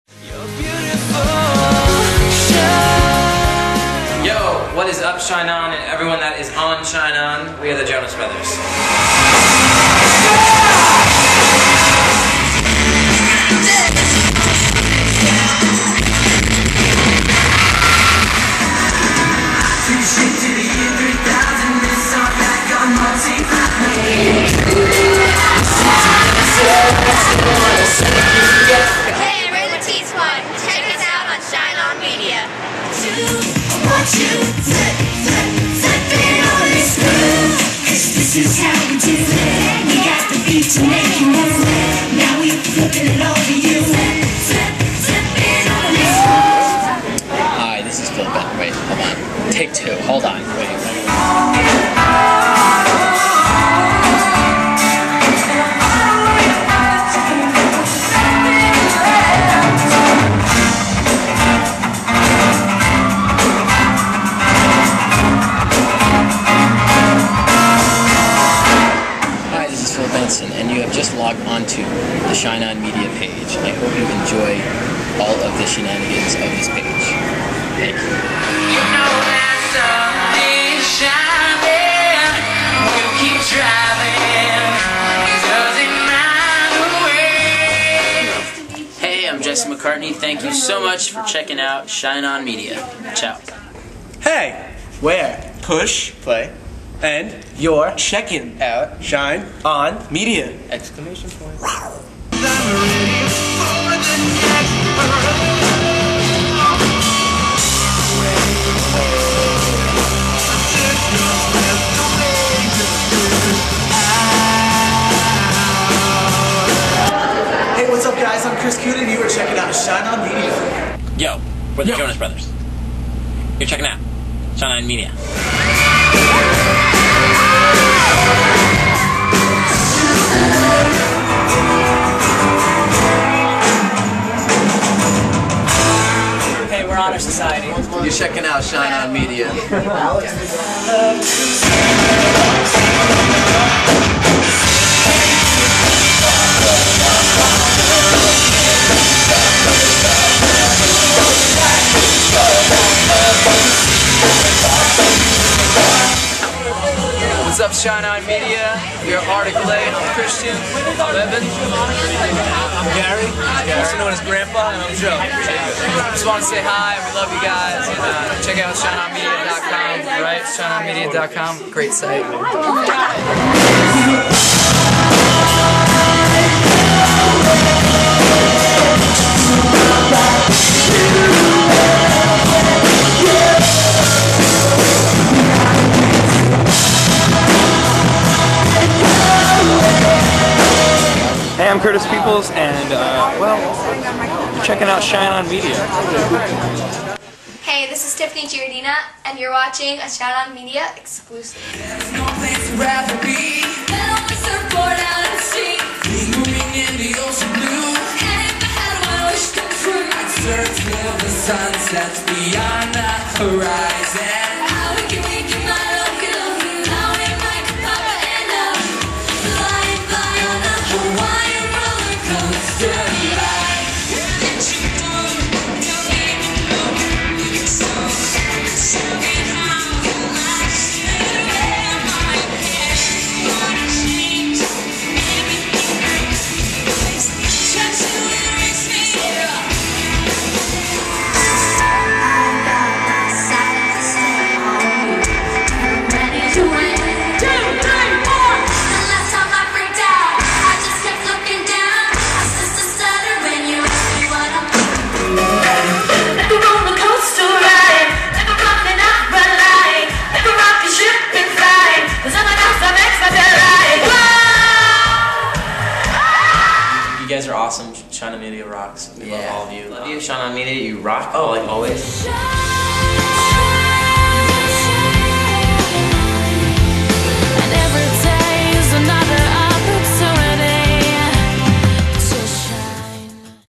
SO-M Promo
Collection of intro clips and video footage. Check out shineon-media.com for videos, interviews, concert photos, etc.
**All video footage filmed exclusively by & for shineon-media.com except the Jesse McCartney Live Footage (Thanks Grace)**